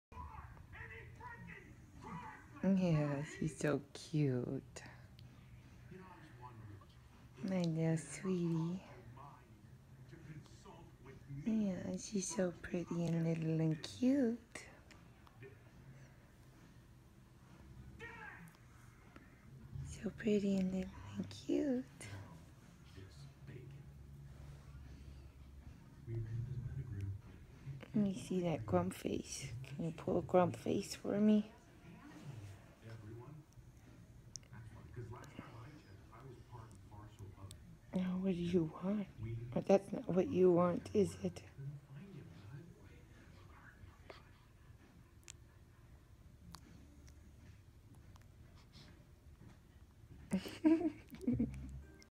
Baby fix! Smiles and grumpy faces while baby sleeps!
I was just fine and happy until mommy put that icky finger in my mouth!